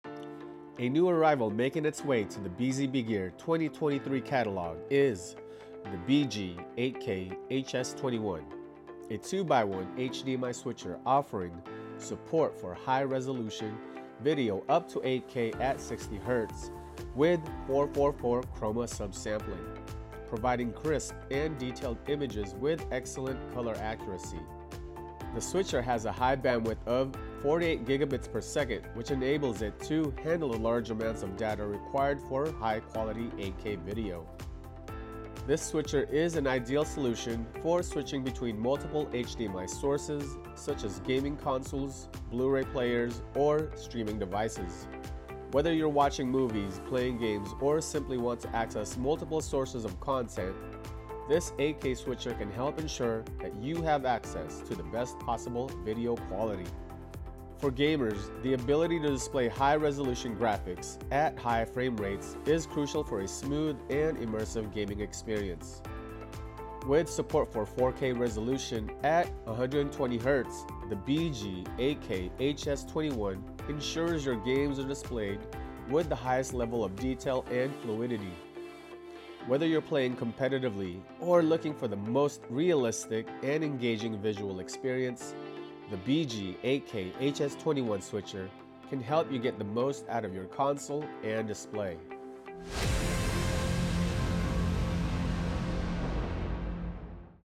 8K HDMI Switcher Renders Immersive Visual Experience - BG-8K-HS21
The 8K 2x1 HDMI 2.1 switcher is a versatile device that allows you to switch between two HDMI sources and display the selected input on a single HDMI display. It offers support for high-resolution video up to 8K at 60Hz with 4:4:4 chroma subsampling, providing crisp and detailed images with excellent color accuracy. Additionally, the switcher has a high video bandwidth of 48Gbps, which enables it to handle the large amounts of data required for high-quality 8K video.